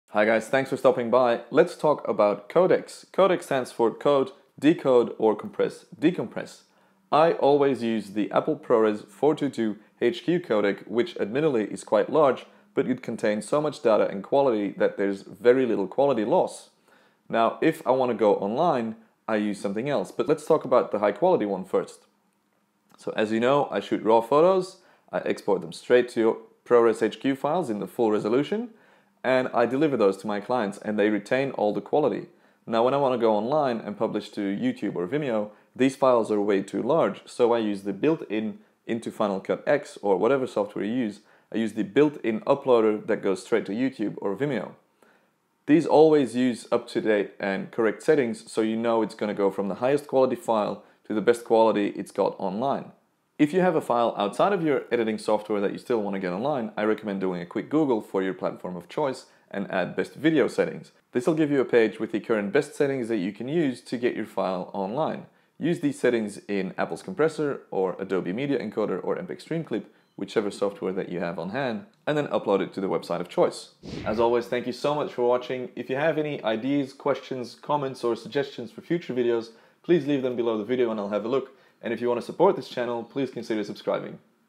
Which codec to use for timelapse and video work
I use the Apple ProRes 422 (HQ) codec for all my master files and edits, while I use Youtube and Vimeo's recommended settings for when I want to publish it online.